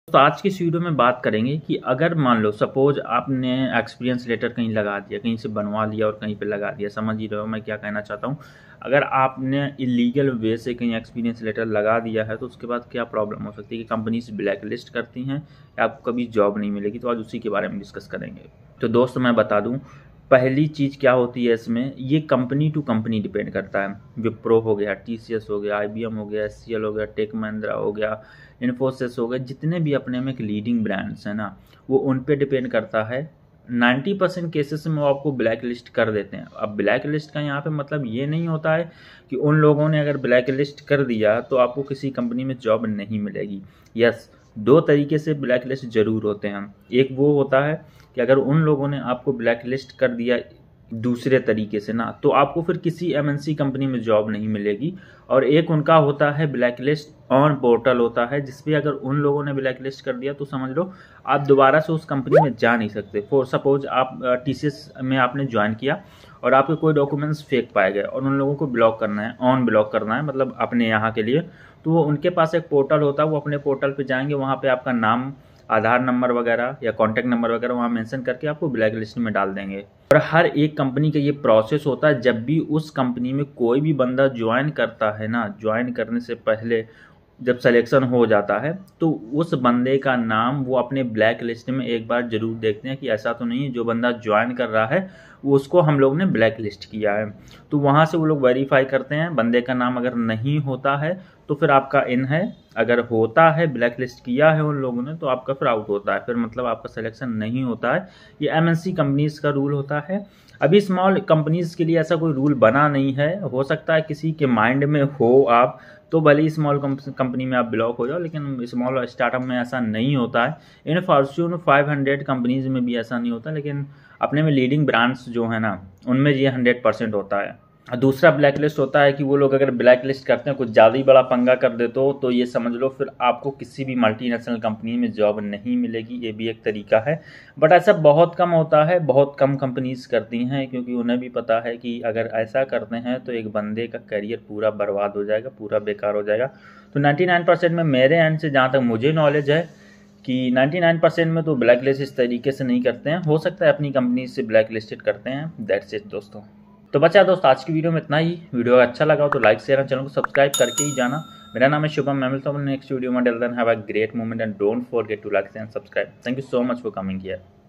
What if you come across a fake experience letter and get caught ?Black list ho Jaoge kya fake letter
Hi Friends .
Maine is video me btaya h ki agr aap fake experience letter lgate ho kisi MNC company me or agr aap pakda jate ho to kya ho skta h kya hamesha ke liye black list ho jate

Thanks.